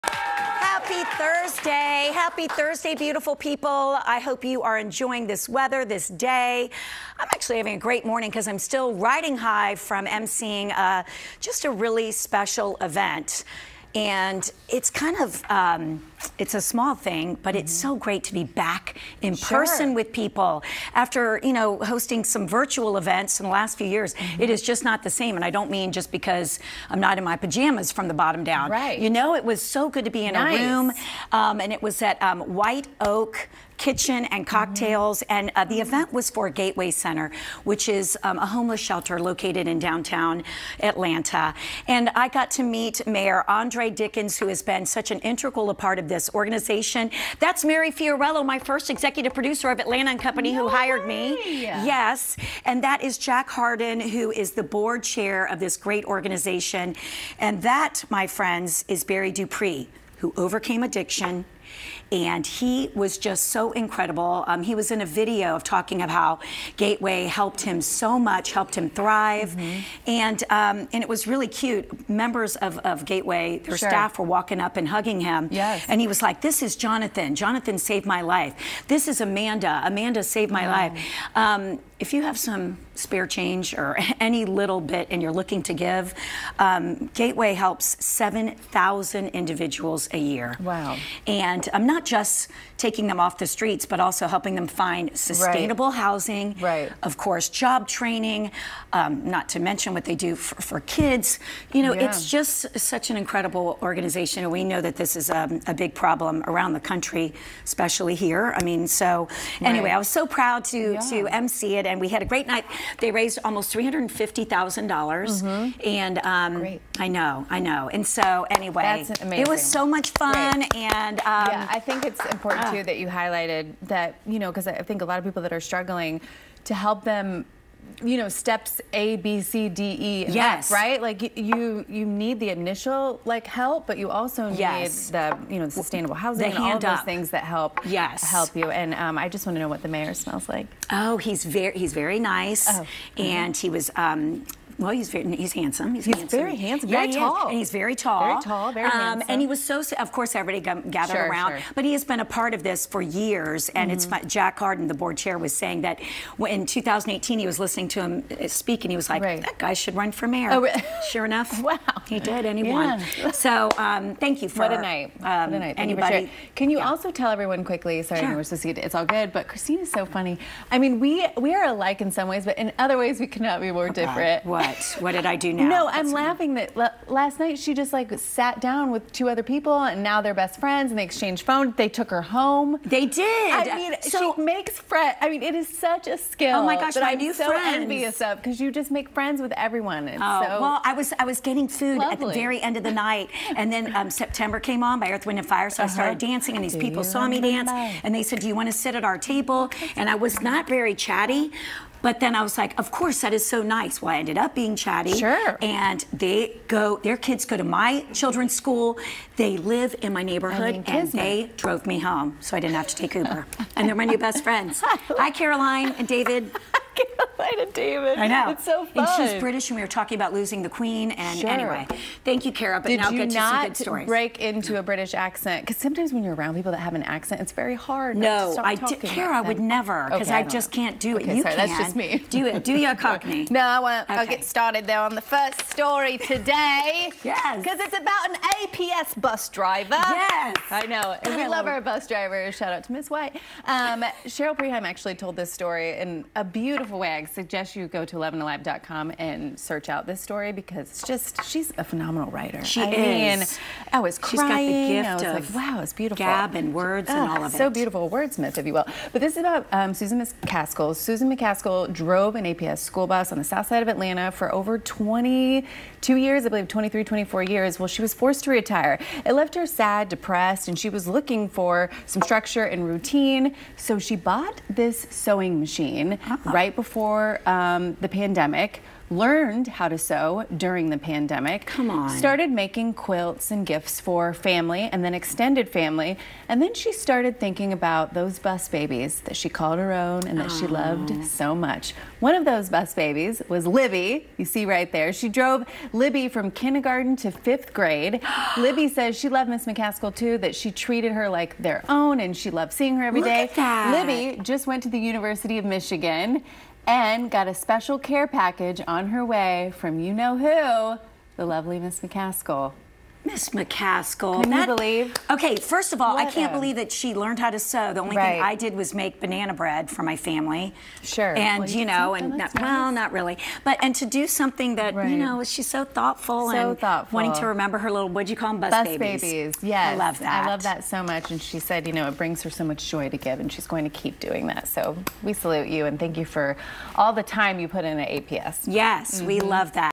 Retired Bus Driver Sews Blankets for Students Headed to College
A former Atlanta Public Schools bus driver sews blankets and pillows for students as they head off to college.